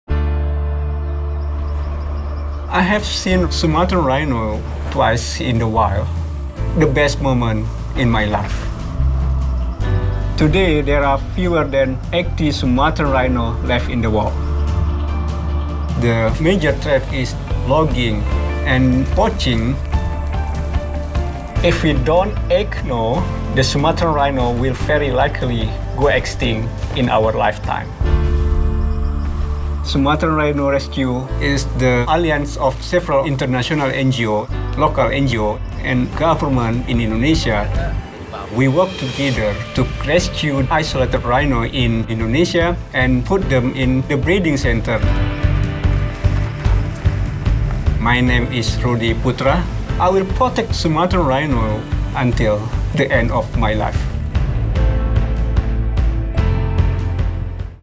Explorers at Work: Rudi Putra
A biologist is trying to save the critically endangered Sumatran rhinoceros in Indonesia.